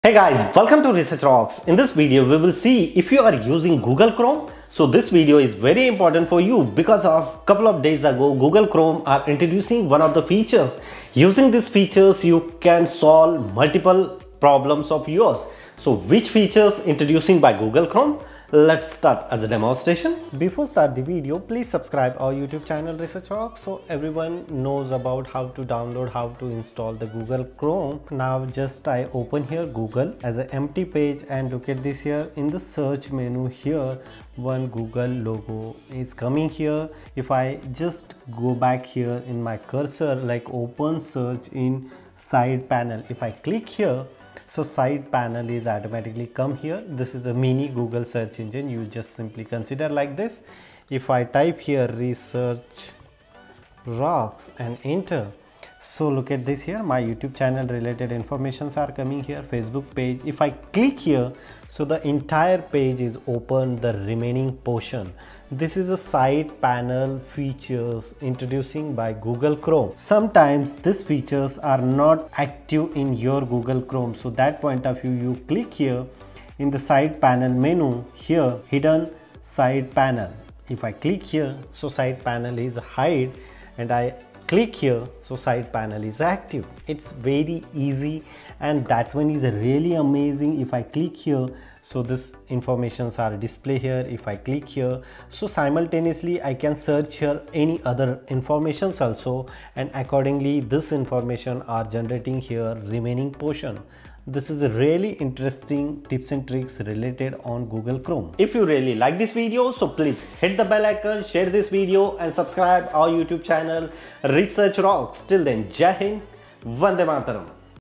How to Enable Side Search Panel in Google Chrome | Google Chrome new Tips and Tricks
We'll see in this video, With the help of an experimental flag, this recently added capability can now be made available in Chrome's stable release. You can view a page and the search results simultaneously with side search, and you can also view a page in your main browser window without switching between them or losing your search results. Side search makes it simple to access your most recent Google search results page embedded in a browser side panel.

About : Research Rocks is a YouTube Channel, where you will find technological and Tech videos, New Video Posted Everyday.